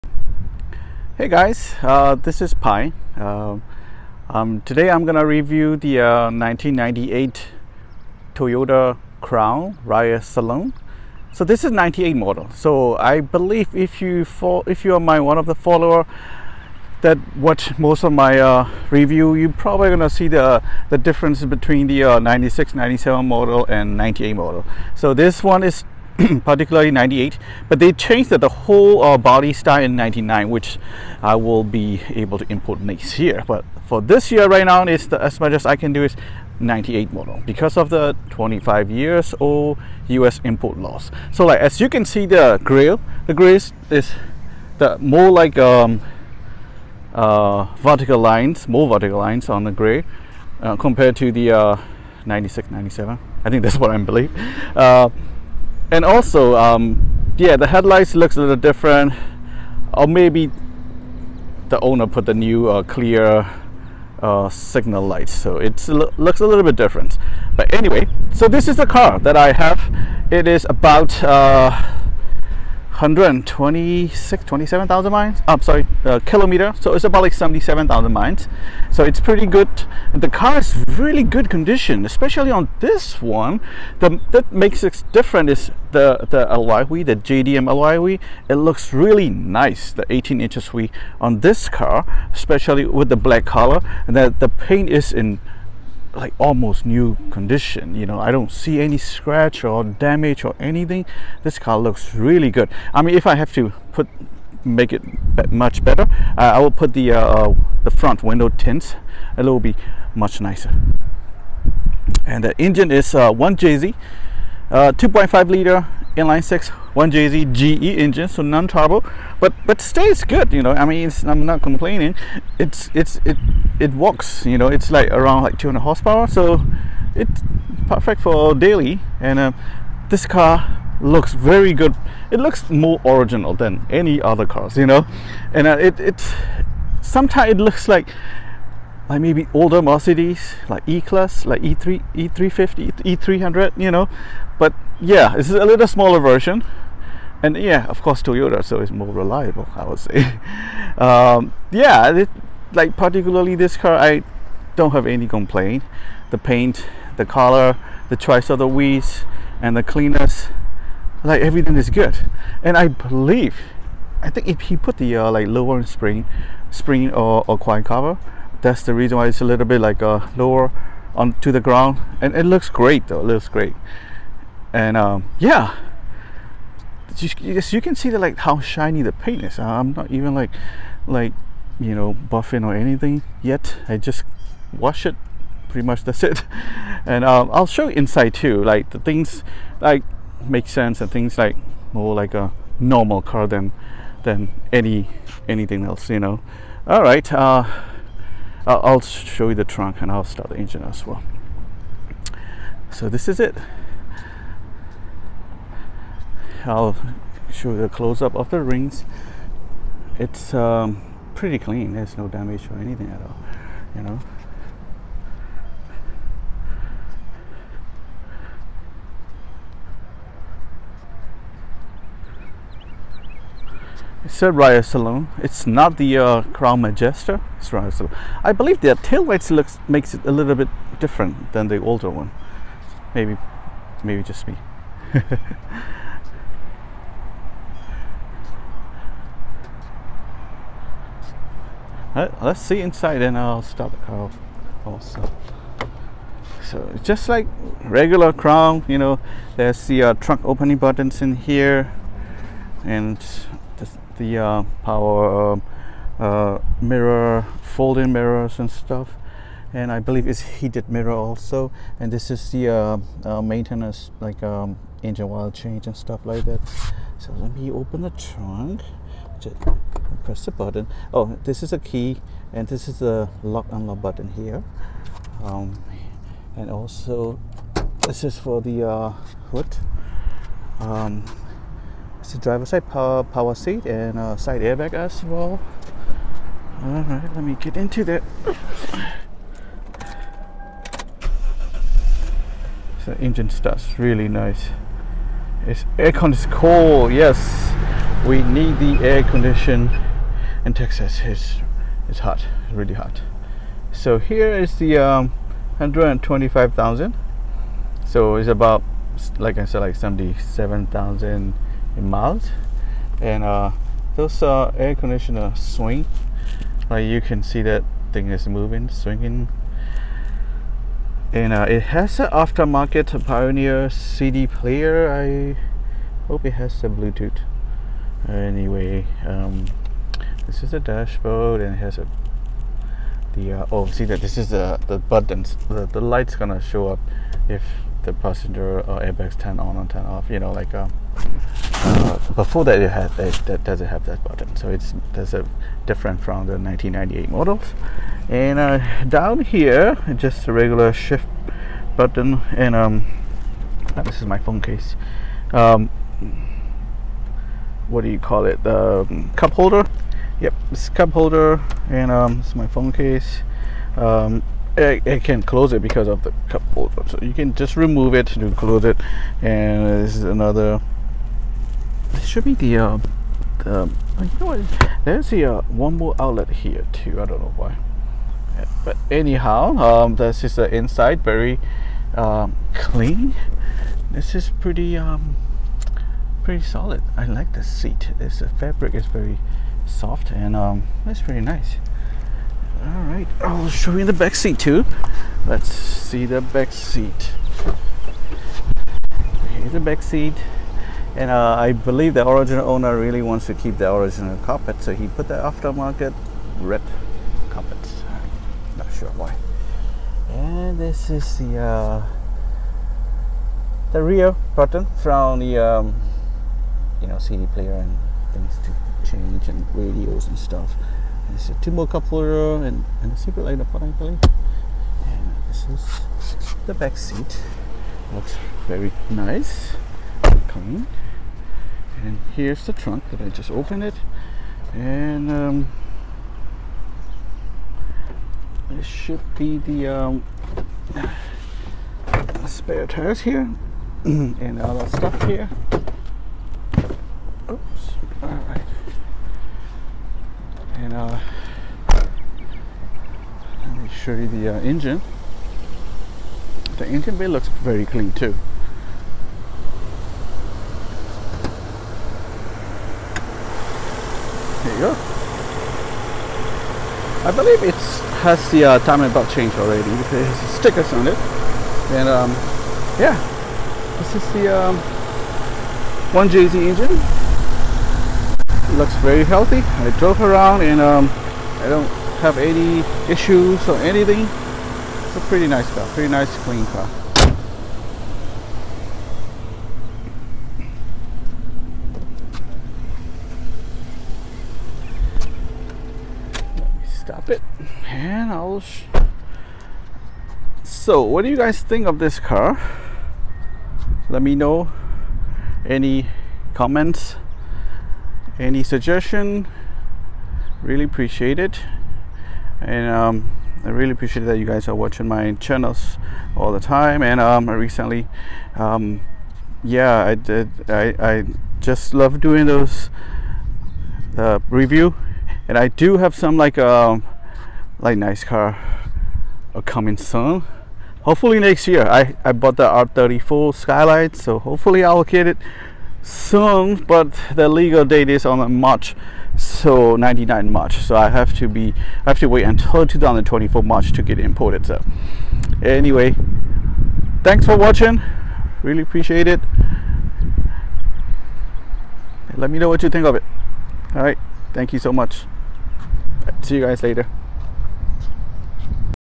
1998 Toyota Crown Royal Saloon G with 1JZGE 2.5L inline six engine. Only 77k miles. RHD JDM
Super clean VIP ready 1998 Toyota Crown Royal Saloon G with 1JZGE 2.5L inline six engine. Only 77k miles. RHD JDM